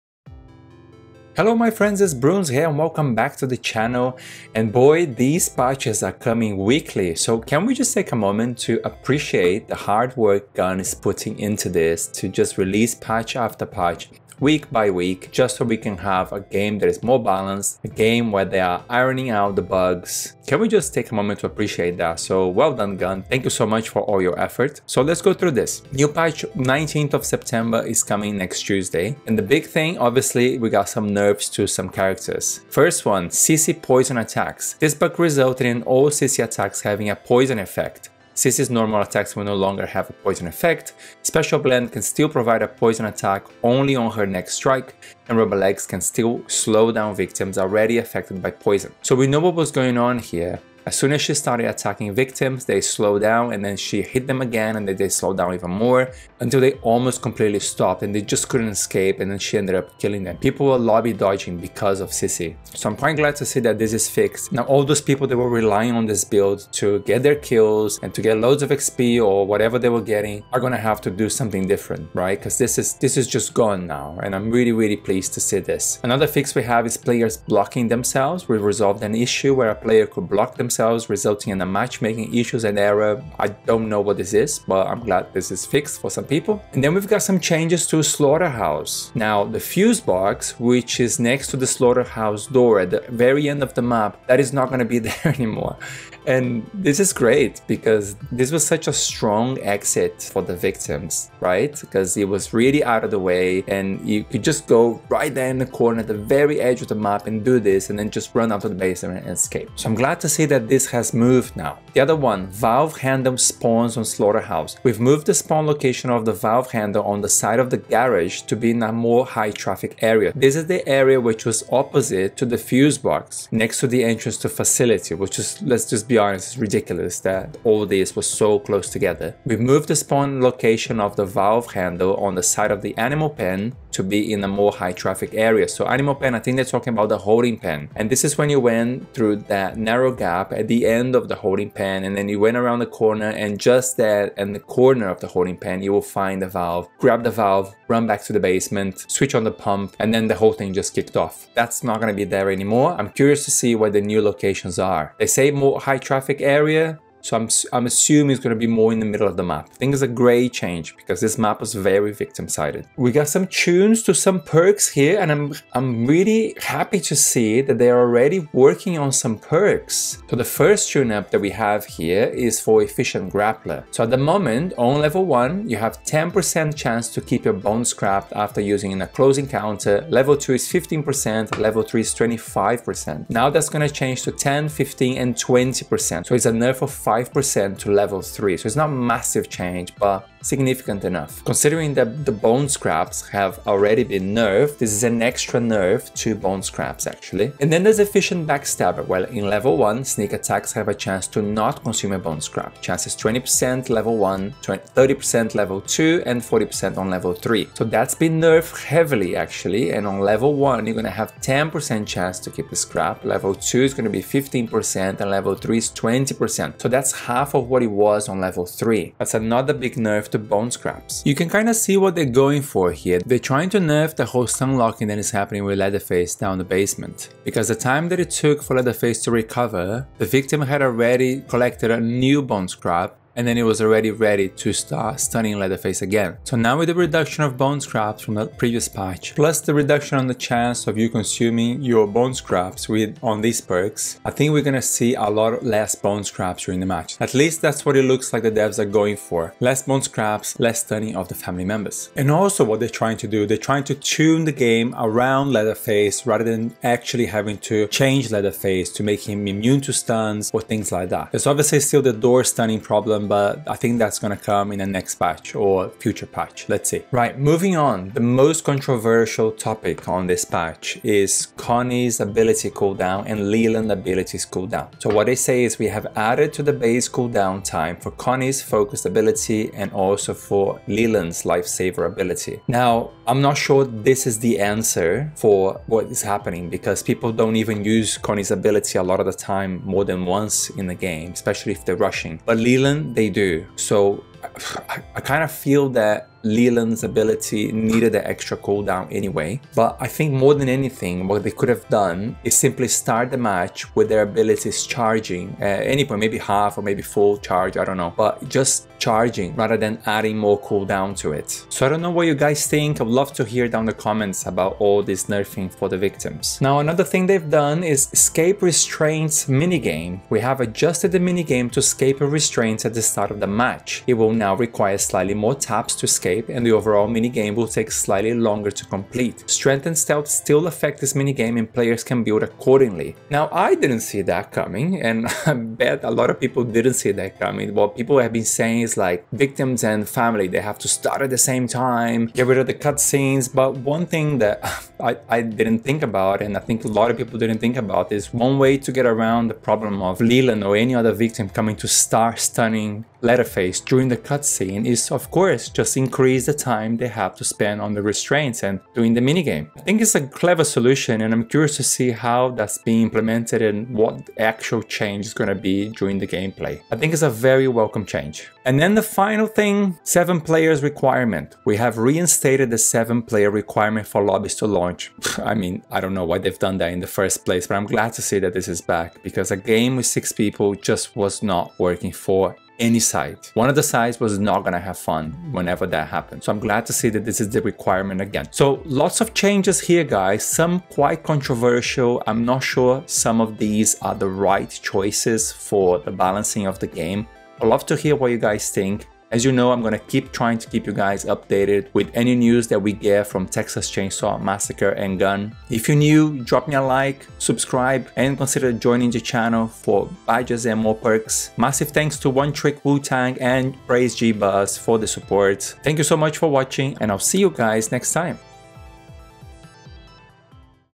TCM PATCH Notes, NERF to Connie, Leland and FIX to Sissy | Texas Chainsaw Massacre Patch
It has happened. The big fix to Sissy, and controversial nerf to Connie and Leland. Plus changes to Slaughterhouse map exits, perk nerfs, and much more

Big shoutout to the members of the channel:
🔝 Praise GBus
🔝 1TrickWootang